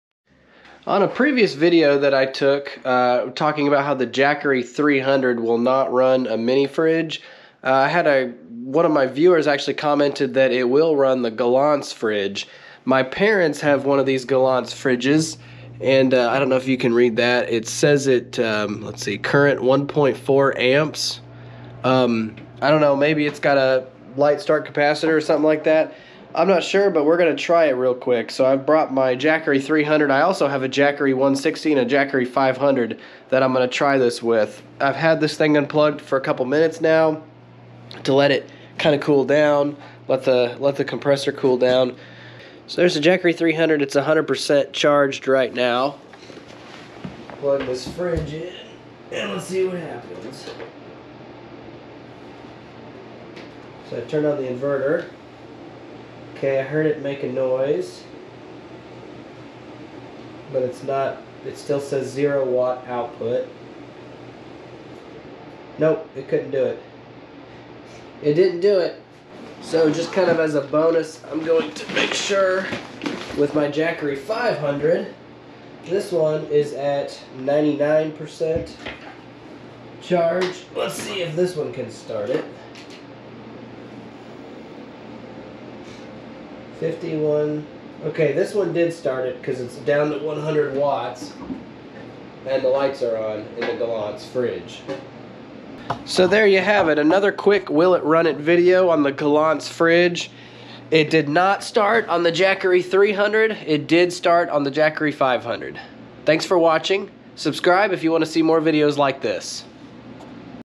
Can a Jackery 300 or 500 run a mid-sized Galanz Fridge?
My hunt continues to see if I can find a 120v fridge that the Jackery 300 can start. In this video I try to run a Galanz fridge on both the Jackery 300, and the Jackery 500.